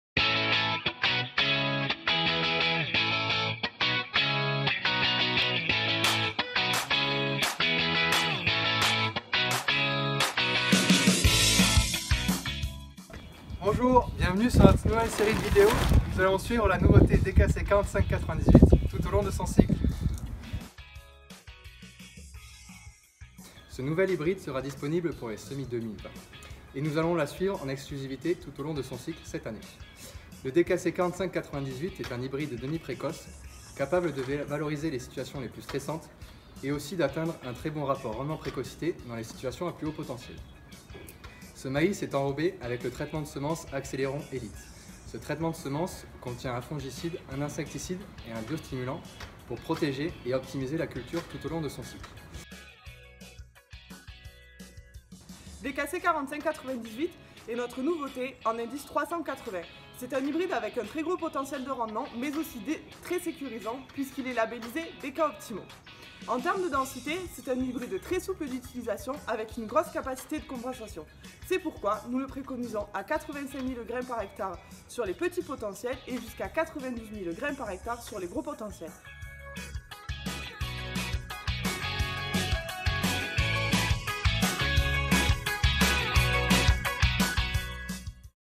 OPENTHEDKTEST : DKC4598, la nouveauté !
Bienvenue sur notre nouvelle série de vidéos « OPENTHEDKTEST », dans ces vidéos nous allons suivre notre nouvelle hybride pour les semis 2019 : le DKC4598, du semis à la récolte !

Pour en savoir plus sur notre nouveau traitement de semences ACCELERON et son biostimulant le B360

Pour plus d’information, n’hésitez pas à contacter votre expert Dekalb !